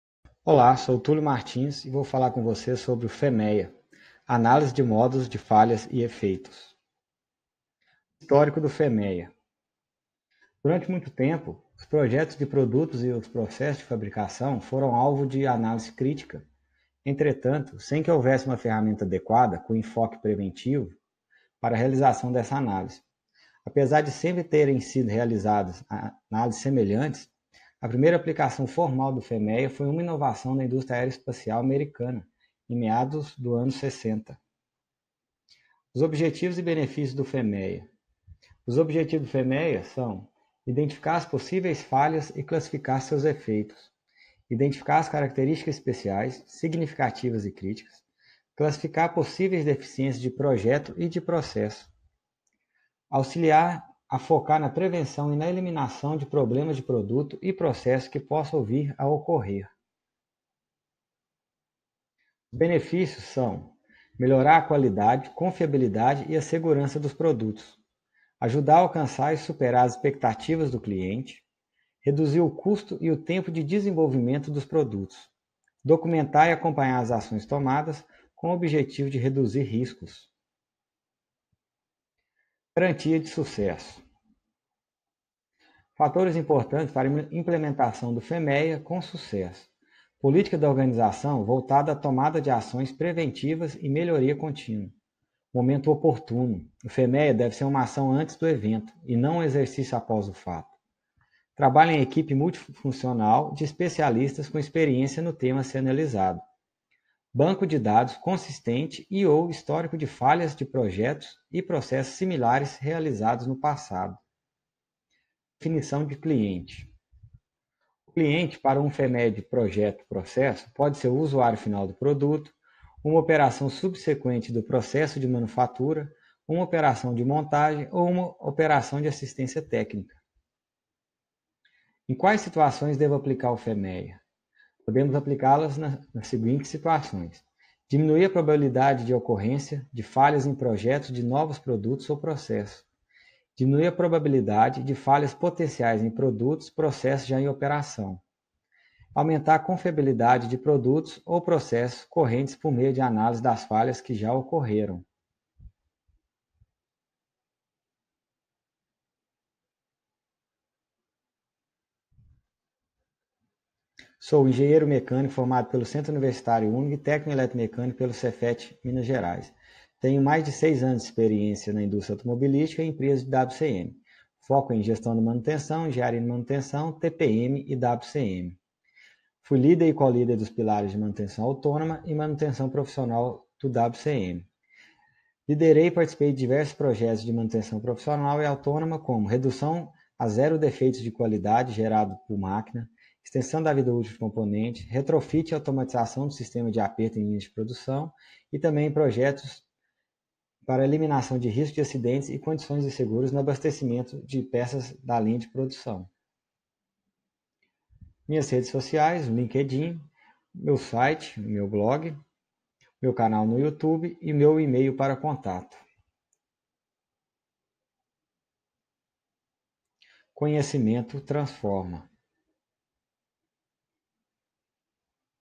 O que é FMEA? Análise de Modos de Falhas e Efeitos!
FMEA - Análise de Modos de Falhas e Efeitos
Failure Mode and Effect Analysis

É uma metodologia que objetiva avaliar e minimizar riscos por meio de análises das possíveis falhas (determinação da causa, efeito e risco de cada tipo de falha), e implementar ações para aumentar a confiabilidade.

O FMEA tem por objetivo identificar, delimitar e descrever as não conformidades (modo da falha) geradas pelo processo e seus efeitos e causas, para através de ações de prevenção poder diminuí-los ou eliminá-los.

FMEA DE PRODUTO: na qual são considerados as falhas que poderão ocorrer com o produto dentro das especificações do projeto. É comumente denominada também de FMEA de Projeto.

FMEA DE PROCESSO: são consideradas as falhas no planejamento e execução do processo, objetivo de evitar falhas do processo, tendo como base as não conformidades do produto com as especificações do projeto.

Podemos aplica-los nas seguintes situações:

- Diminuir a probabilidade da ocorrência de falhas em projetos de novos produtos ou processos;

- Diminuir a probabilidade de falhas potenciais em produtos, processos já em operação;

- Aumentar a confiabilidade de produtos ou processos correntes por meio de análises das falhas que já ocorreram;